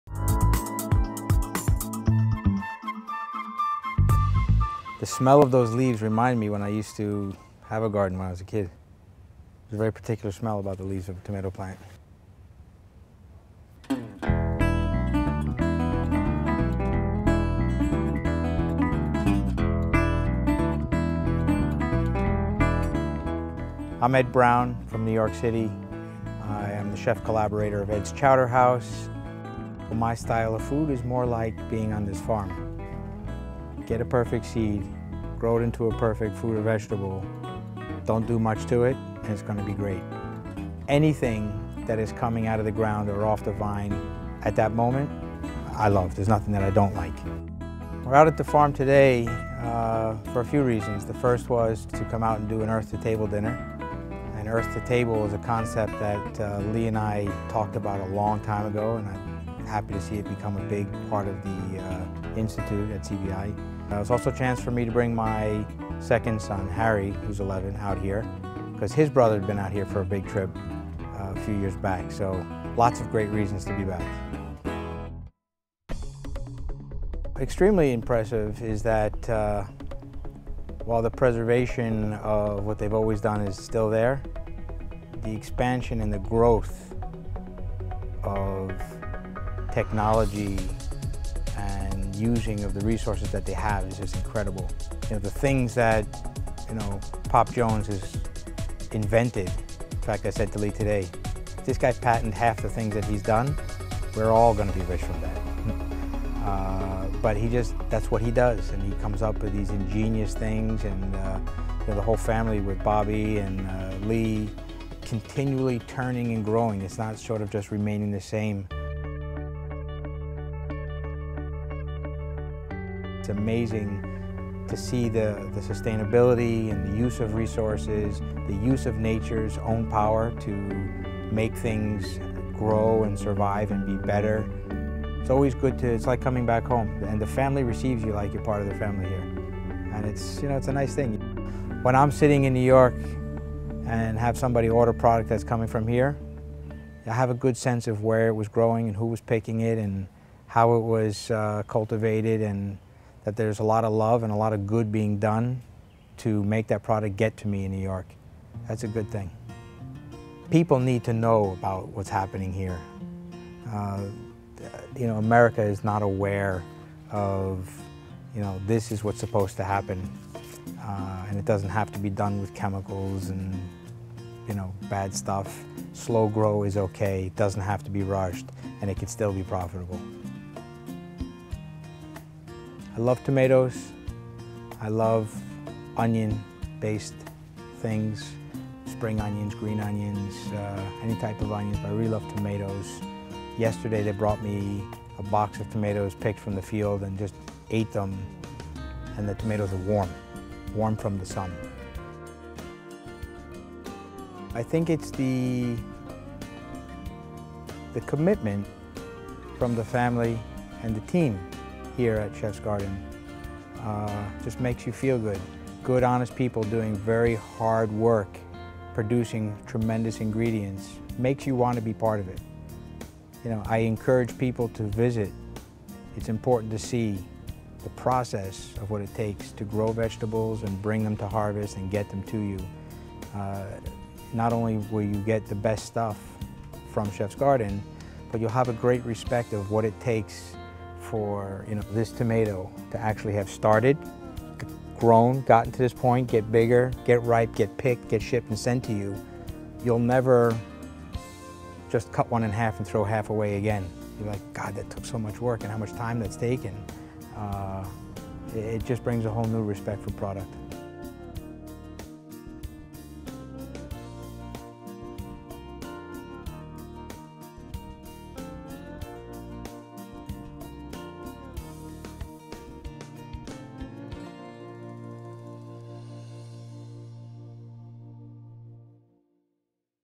Chef Ed Brown: Respect the Tomato
Chef/collaborator Ed Brown garners respect for tomatoes and over 600 varieties of vegetables, herbs, and microgreens while visiting The Chef's Garden, a sustainable family farm in Huron, Ohio. He was on the farm recently preparing an Earth to Table dinner at The Culinary Vegetable Institute in Milan, Ohio, and appeared on last night's episode of Iron Chef.